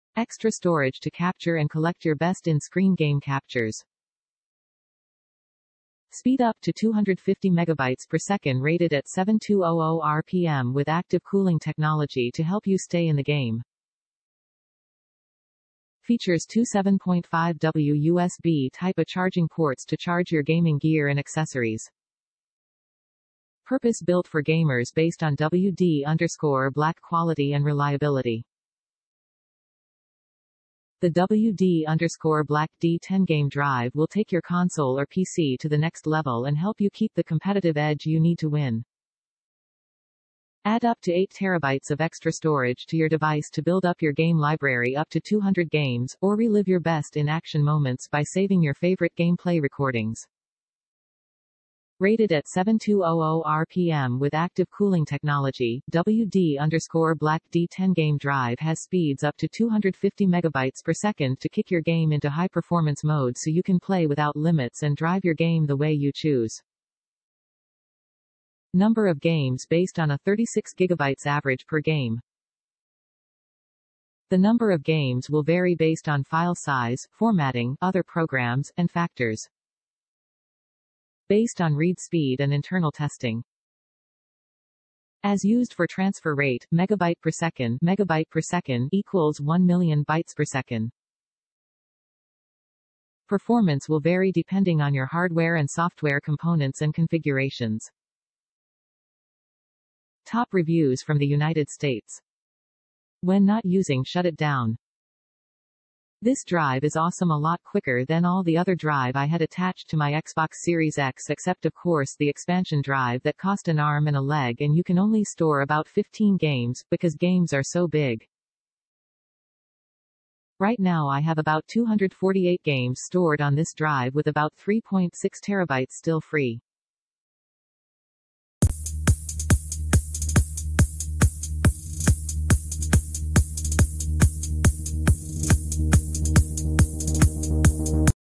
WD_BLACK 8TB D10 Game Drive - Portable External Hard Drive HDD Compatible with Playstation, Xbox, PC
Extra storage to capture and collect your best in-screen game captures.
Speed up to 250 MB/s rated at 7200RPM with active cooling technology to help you stay in the game.
Features two 7.5W USB Type-A charging ports to charge your gaming gear and accessories.
Purpose-built for gamers based on WD_Black quality and reliability.
The wd_black D10 game Drive will take your console or PC to the next level and help you keep the competitive edge you need to win. Add up to 8TB of extra storage to your device to build up your game library up to 200 games, or Relive your best in-action moments by saving your favorite game play recordings. Rated at 7200 RPM with Active cooling technology, wd_black D10 game Drive has speeds up to 250MB/s to kick your game into high-performance mode so you can play without limits and drive your game The way you choose. | Number of games based on a 36GB average per game. The number of games will vary based on File size, formatting, other programs, and factors. Based on read speed and internal testing. As used for transfer rate, megabyte per second (MB/s) = one million bytes per second. Performance will vary depending on your hardware and software components and configurations.

When not using shut it down
This drive is awesome a lot quicker then all the other drive i had attached to my xbox series x except of course the expansion drive that cost an arm and a leg and you can only store about 15 games, because games are so big. Right now i have about 248 games stored on this drive with about 3.6 terabytes still free